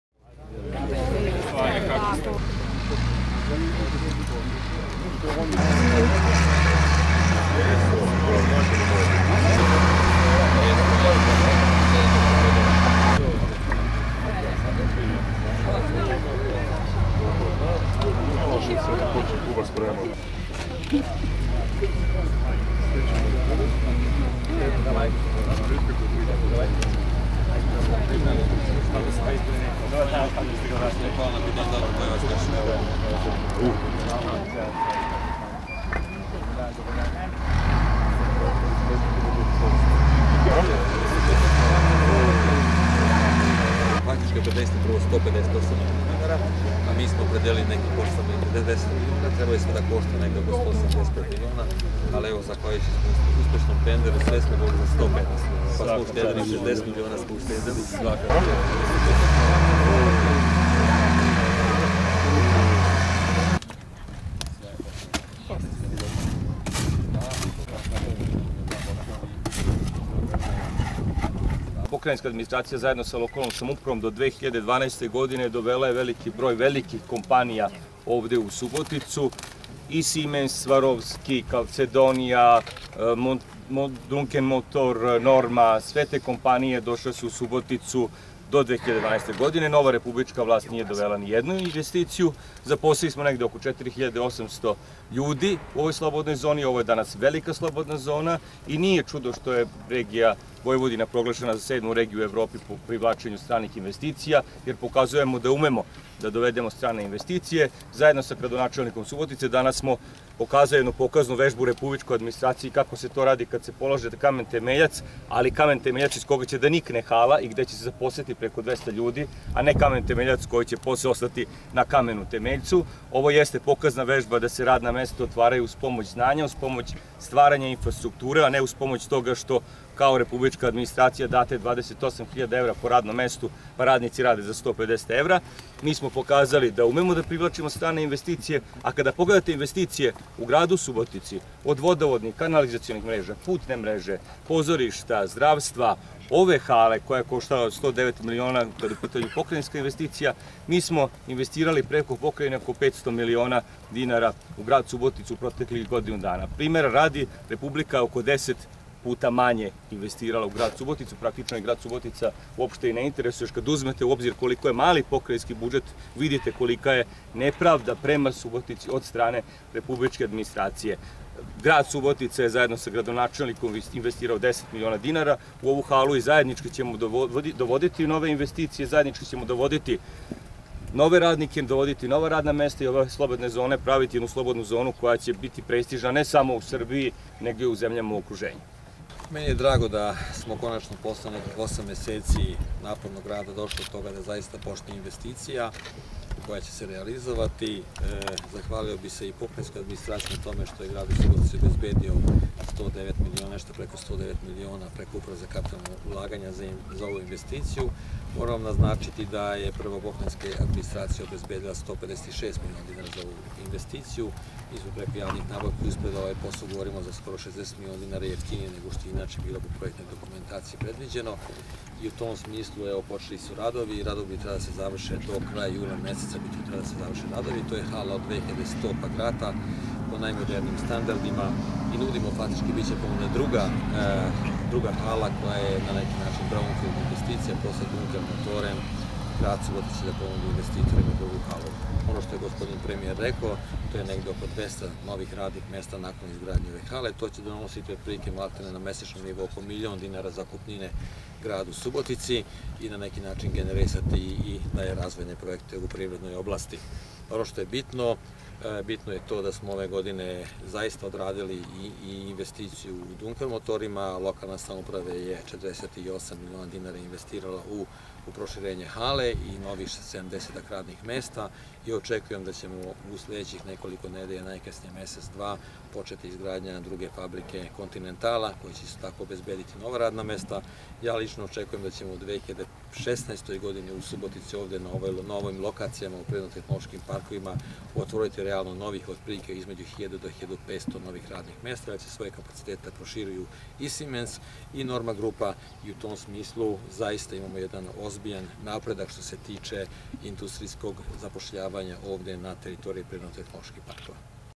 Polozen kamen temeljac za izgradnju nove hale u Malom Bajmoku
super-info.rs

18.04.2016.

Položen kamen temeljac za izgradnju nove hale u Malom Bajmoku

Predsednik Pokrajinske vlade Bojan Pajtić i gradonačelnik Subotice Jene Maglai, prisustvovali su polaganju kamena temeljca za izgradnju nove proizvodne hale u Industrijskoj zoni „Mali Bajmok“, ukupne površine 2.100 kvadrata.

Nova hala u za čiju je izgradnju Uprava za kapitalna ulaganja opredelila 109 miliona, a Grad 10 miliona dinara, trebala bi da bude završena do juna, a u njoj će biti zaposleno oko 200 ljudi.